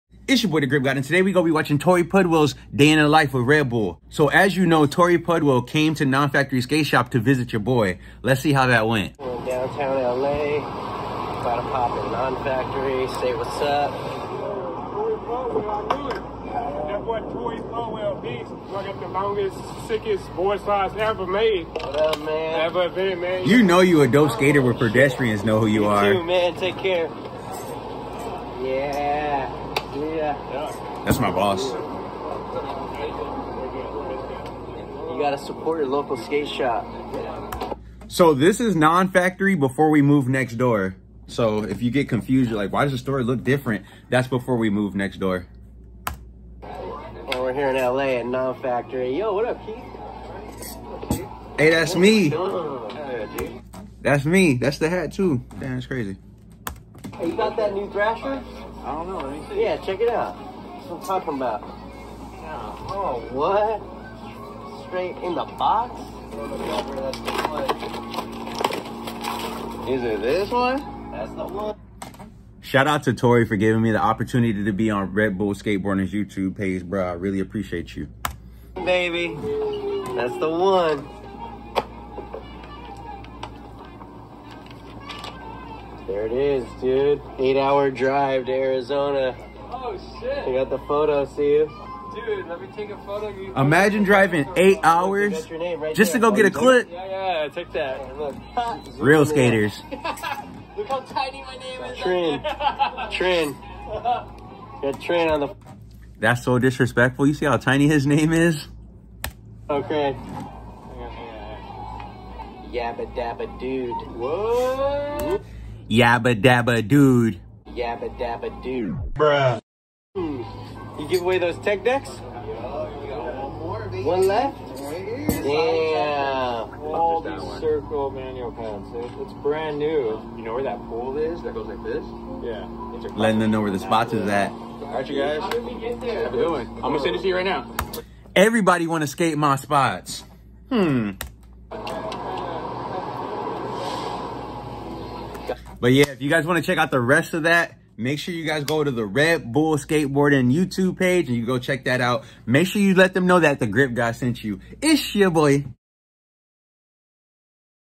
Coffee Clips “ Torey Pudwill “
Grab your coffee and let’s watch Torey pudwills Day in the life with Red Bull skateboarding